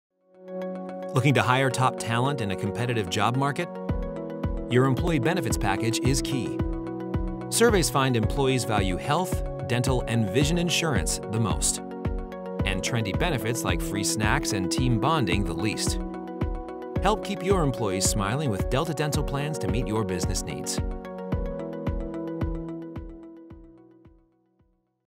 The most and least valued employee benefits
If you’re looking to hire top talent in a competitive job market, your employee benefits package could be key.